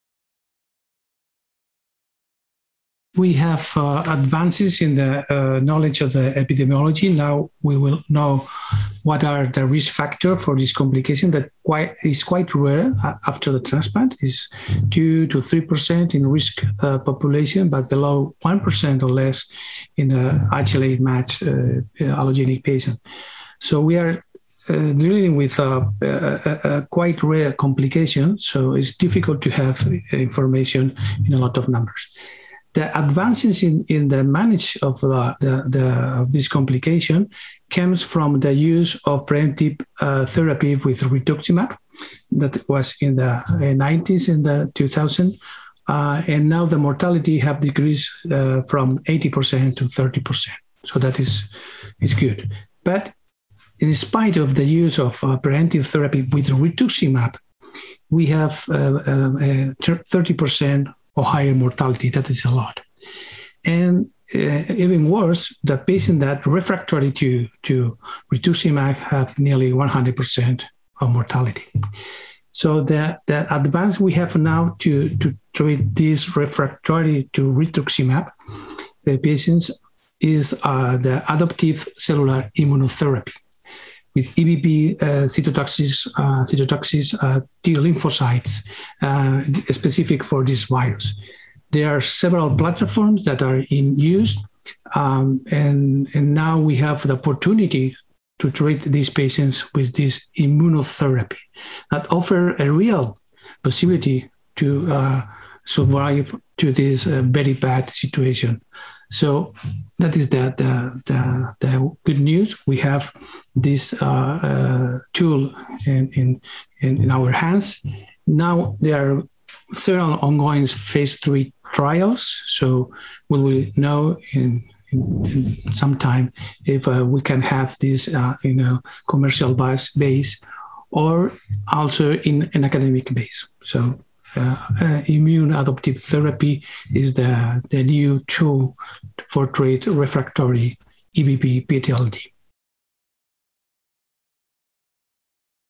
Advances in EBV-PTLD
Rafael de la Cámara, MD, Hospital de la Princesa, Madrid, Spain, shares an update on recent advances regarding the treatment of Epstein-Barr virus–positive post-transplant lymphoproliferative disease (EBV-PTLD). Whilst the use of pre-emptive therapy with rituximab significantly decreases the EBV-PTLD mortality rate, it is not a suitable treatment option for patients who are rituximab-refractory. Dr Cámara reports that recently adoptive cellular immunotherapy with Epstein-Barr virus (EBV)-specific cytotoxic T-lymphocytes (CTL) has enabled the treatment of these patients. This interview took place at the 4th Annual Meeting of the International Academy for Clinical Hematology (IACH), which took place virtually in 2021.